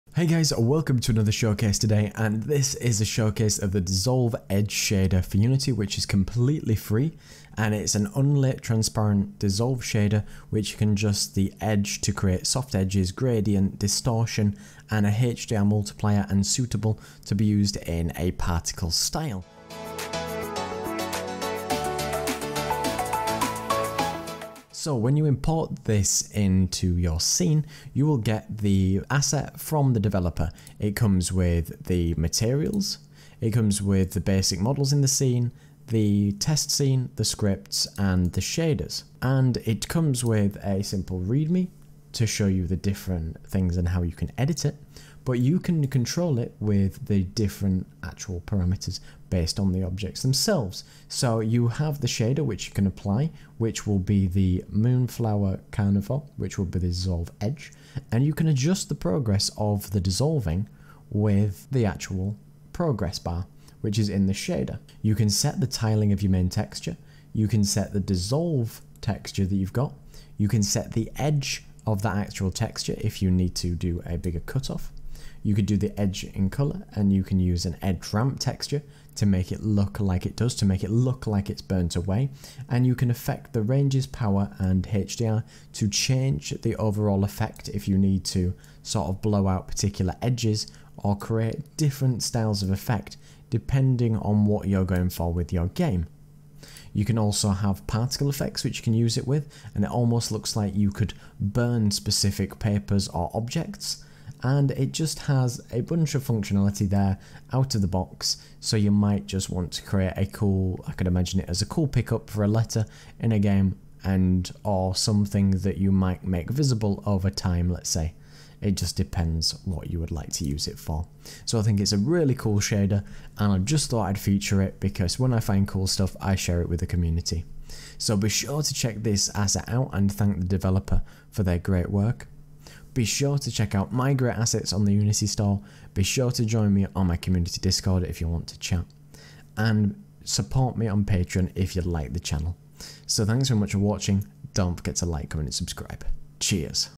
FREE DISSOLVE Edge SHADER in Unity!
This is a showcase of a FREE Dissolve shader for Unity, which allows you burn or dissolve the edge of a texture and even works with a particle system!